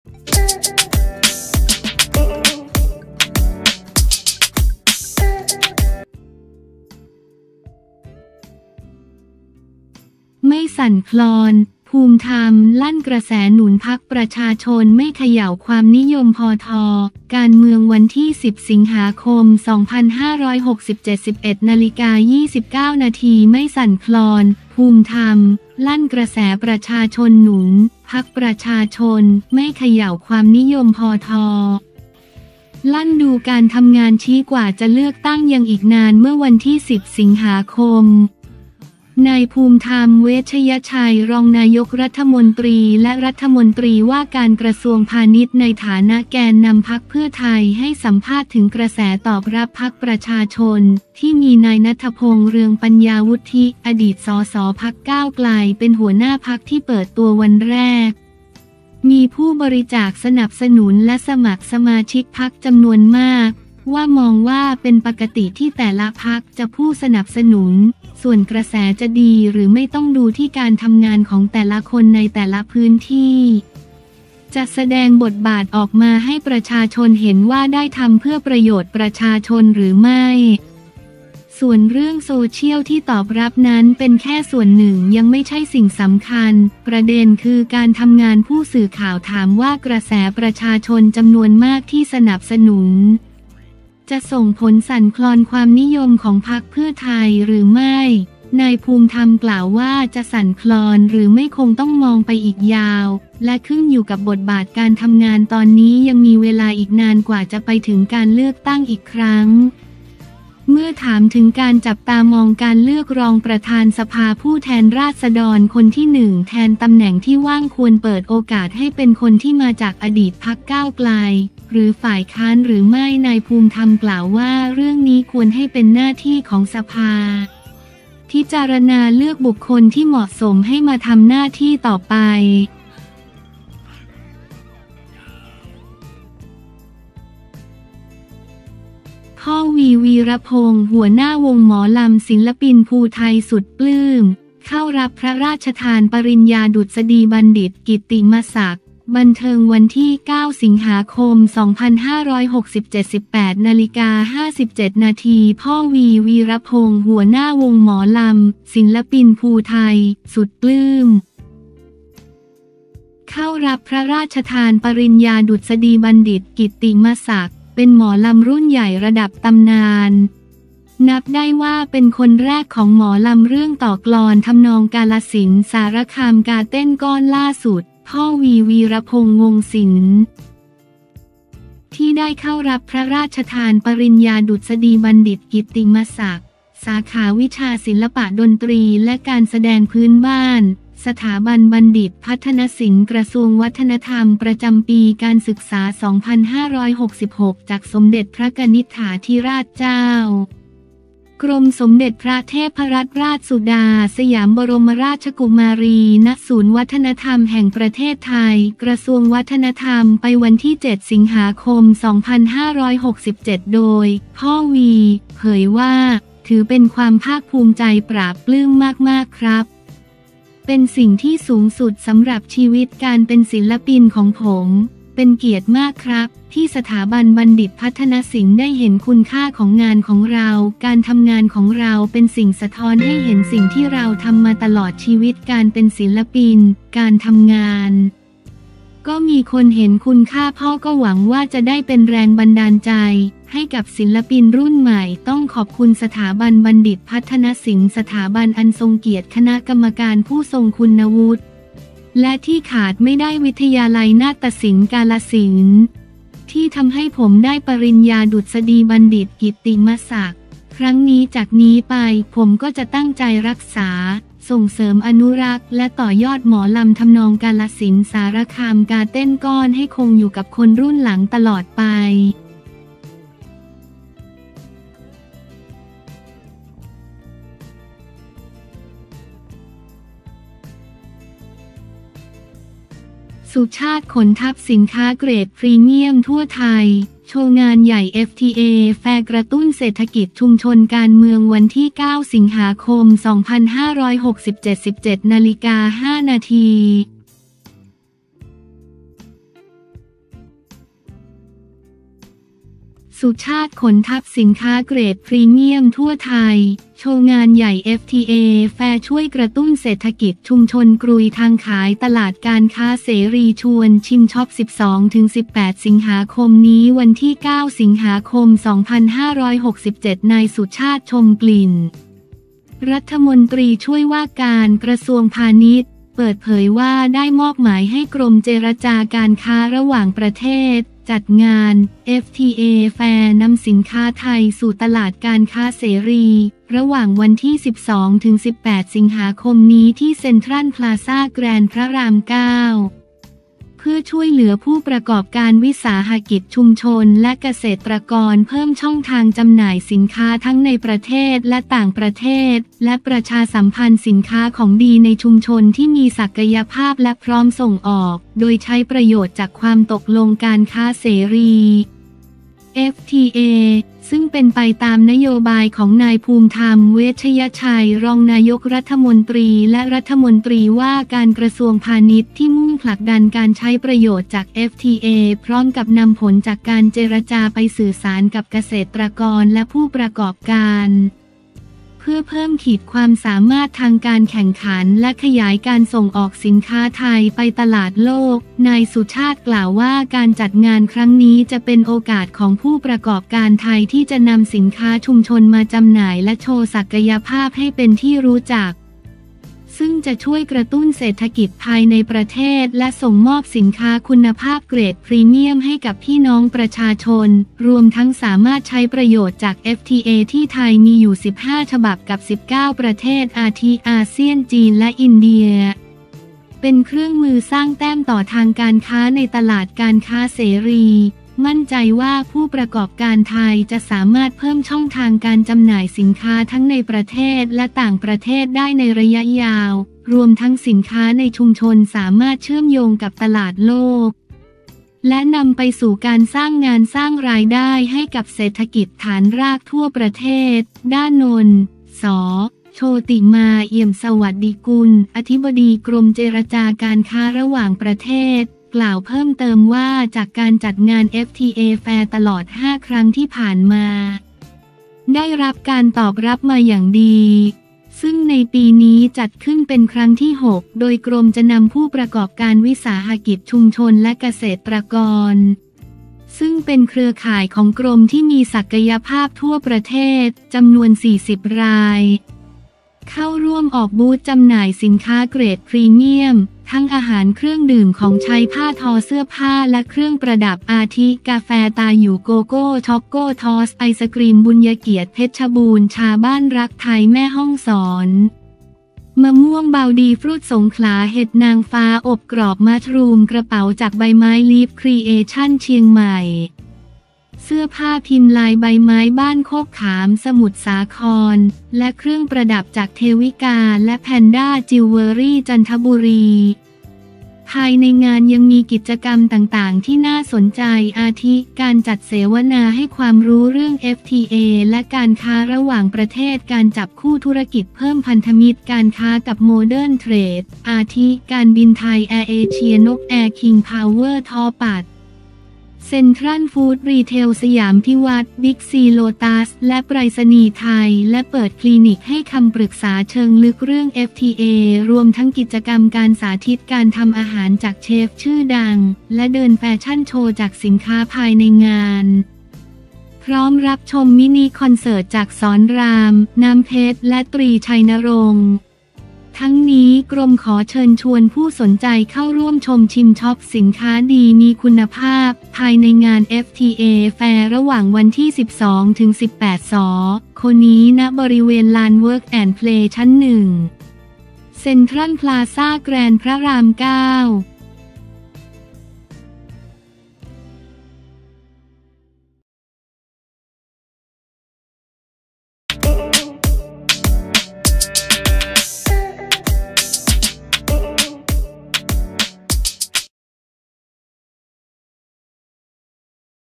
ไม่สั่นคลอน! ภูมิธรรม ลั่นกระแสหนุน พรรคประชาชน ไม่เขย่าความนิยมพท
10 ส.ค. 2567
11:29 น.
ไม่สั่นคลอน! “ภูมิธรรม”ลั่นกระแสปชช.หนุน “พรรคประชาชน” ไม่เขย่าความนิยมพท. ลั่น ดูการทำงาน ชี้ กว่าจะเลือกตั้งยังอีกนาน

เมื่อวันที่10 ส.ค. นายภูมิธรรม เวชยชัย รองนายกรัฐมนตรี และรมว.พาณิชย์ ในฐานะแกนนำพรรคเพื่อไทย ให้สัมภาษณ์ถึงกระแสตอบรับพรรคประชาชน ที่มีนายณัฐพงษ์ เรืองปัญญาวุฒิ อดีตสส.พรรคก้าวไกล เป็นหัวหน้าพรรค ที่เปิดตัววันแรก มีผู้บริจาคสนับสนุนและสมัครสมาชิกพรรคจำนวนมาก ว่า มองว่าเป็นปกติที่แต่ละพรรคจะผู้สนับสนุน ส่วนกระแสจะดีหรือไม่ต้องดูที่การทำงานของแต่ละคนในแต่ละพื้นที่ จะแสดงบทบาทออกมาให้ประชาชนเห็นว่าได้ทำเพื่อประโยชน์ประชาชนหรือไม่ ส่วนเรื่องโซเชียลที่ตอบรับนั้นเป็นแค่ส่วนหนึ่งยังไม่ใช่สิ่งสำคัญ ประเด็นคือการทำงาน
ผู้สื่อข่าวถามว่ากระแสประชาชนจำนวนมากที่สนับสนุน จะส่งผลสั่นคลอนความนิยมของพรรคเพื่อไทยหรือไม่ นายภูมิธรรม กล่าวว่า จะสั่นคลอนหรือไม่คงต้องมองไปอีกยาว และขึ้นอยู่กับบทบาทการทำงาน ตอนนี้ยังมีเวลาอีกนานกว่าจะไปถึงการเลือกตั้งอีกครั้ง

เมื่อถามถึงการจับตามองการเลือกรองประธานสภาผู้แทนราษฎรคนที่1 แทนตำแหน่งที่ว่าง ควรเปิดโอกาสให้เป็นคนที่มาจากอดีตพรรคก้าวไกล หรือฝ่ายค้านหรือไม่ นายภูมิธรรม กล่าวว่า เรื่องนี้ควรให้เป็นหน้าที่ของสภาฯ พิจารณาเลือกบุคคลที่เหมาะสม ให้มาทำหน้าที่ต่อไป